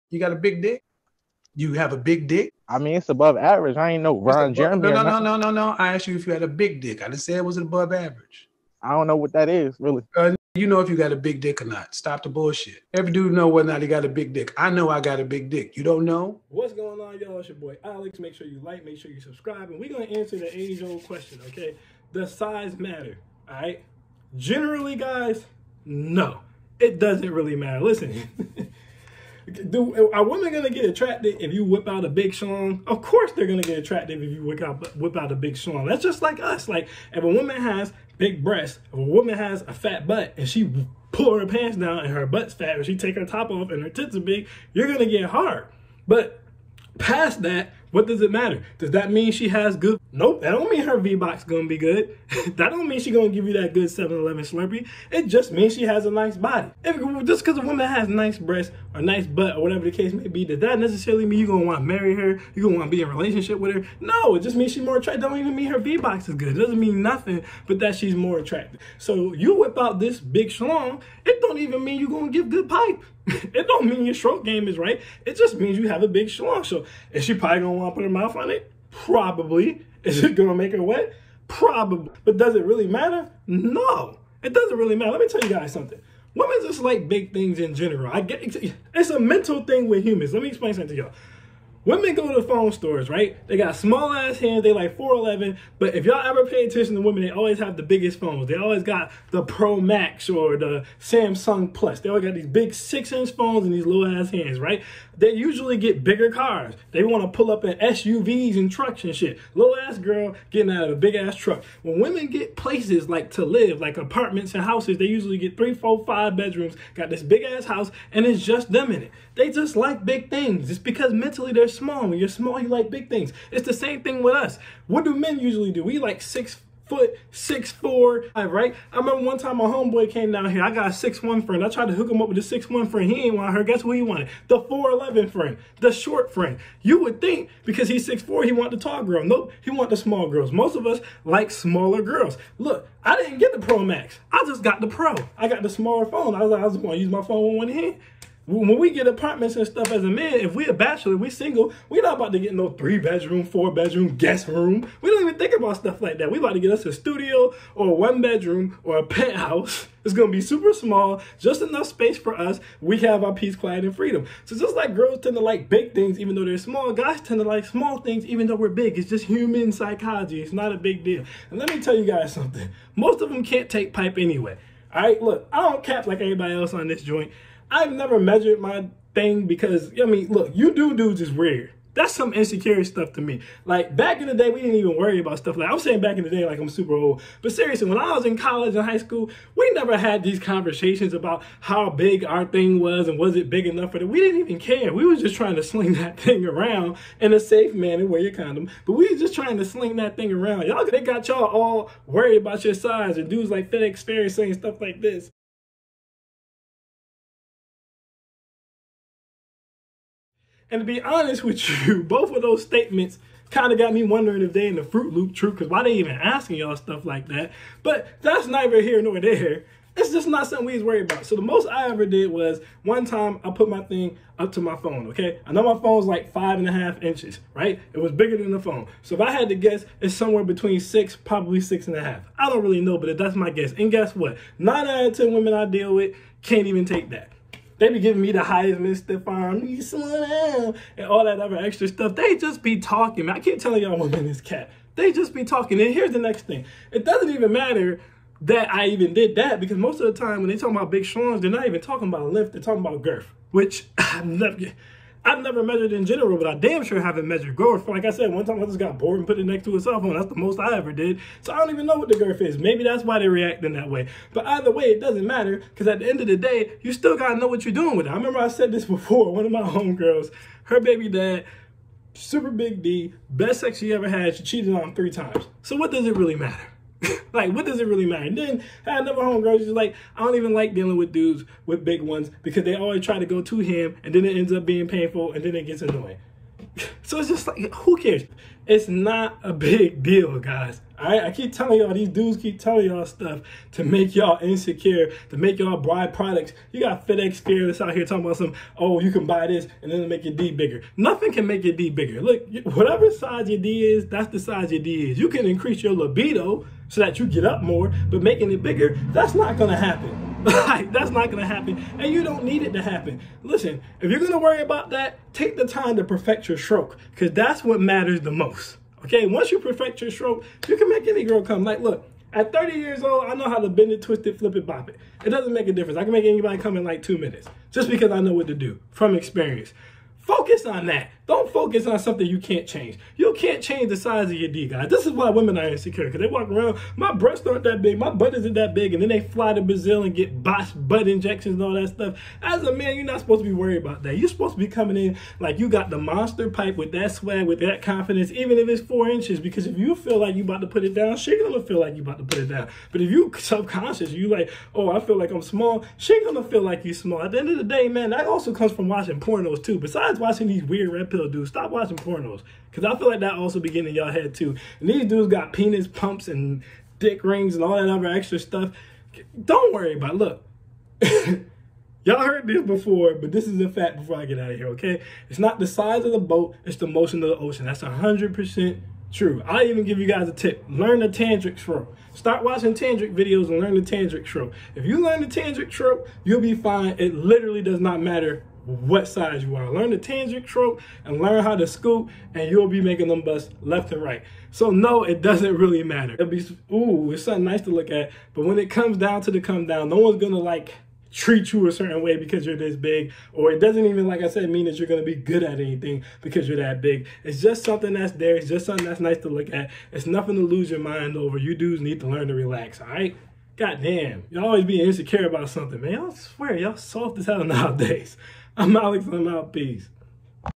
Does size matter?
Alex addresses rather or not 🍆 size matters